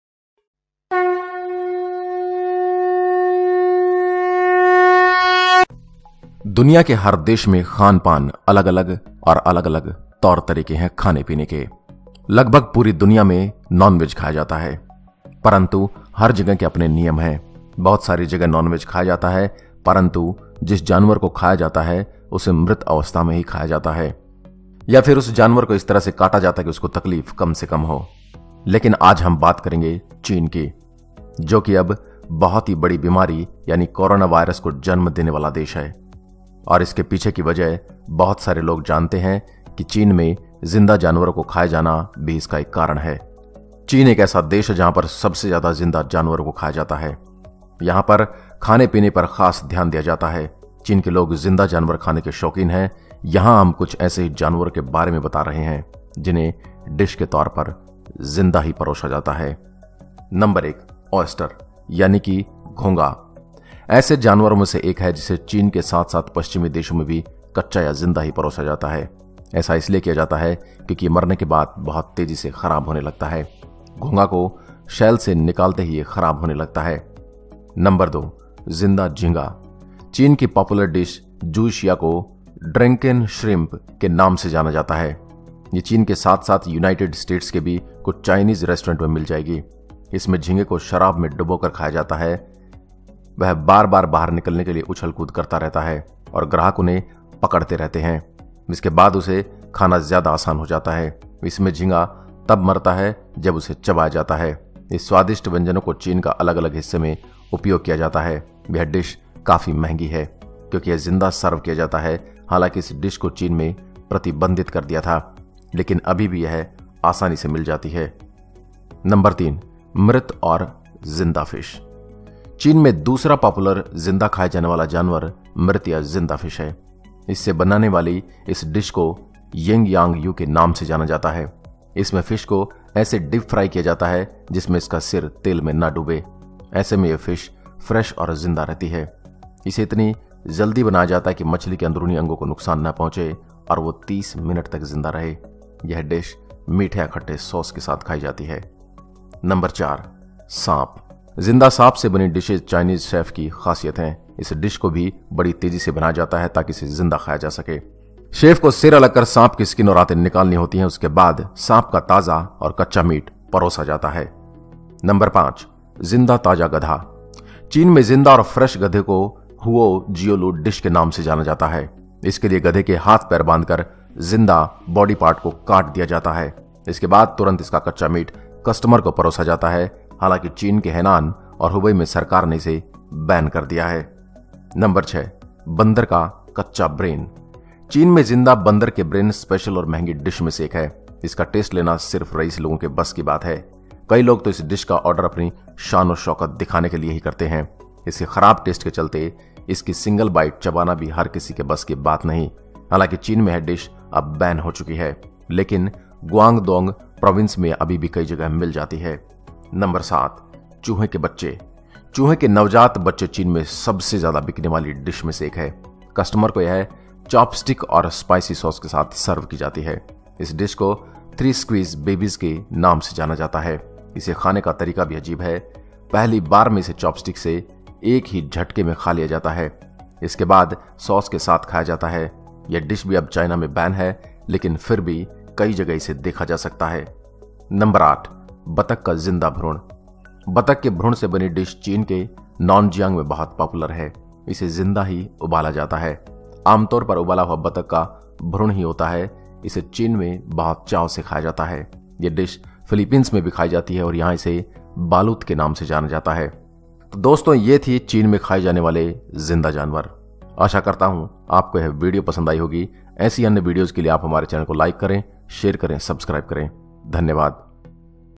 कोरोना और चीन की इस सच्चाई को आप नहीं जानते | TRUTH OF CHINA and Corona
दोस्तों आप सभी जानते हैं कि कोरोनावायरस ने किस प्रकार पूरी दुनिया में अपना कहर बरपा रखा है परंतु बहुत कम लोग जानते हैं इस कोरोनावायरस के जन्म और उसके पीछे की कहानी को चीन के महान शहर में जहां से वायरस चला है उस शहर की हकीकत आपको हैरान कर देगी इस शहर में सबसे ज्यादा जिंदा जानवर को खाया जाता है यहां पर 126 प्रकार के जानवर आपको यहां की मीट मार्केट में मिल जाएंगे जहां पर मुख्यतः चूहे के बच्चे जिंदा गधे जिंदा मछली जिंदा के कड़े जिंदा ऑक्टोपस जिंदा सांप और भी बहुत सारे जानवर जिंदा खाए जाते हैं इन जानवरों में मिलने वाले वायरस या कीटाणु मानव शरीर में प्रवेश करने के बाद मानव शरीर को भी उसके दानों से प्रभावित कर सकते हैं चली आज की वीडियो में हम जानते हैं कि किस किस जानवर को यहां के लोग जिंदा खाना पसंद करते हैं इस वीडियो को अधिक से अधिक शेयर करें ताकि चीन की सच्चाई पूरी दुनिया को पता लग सके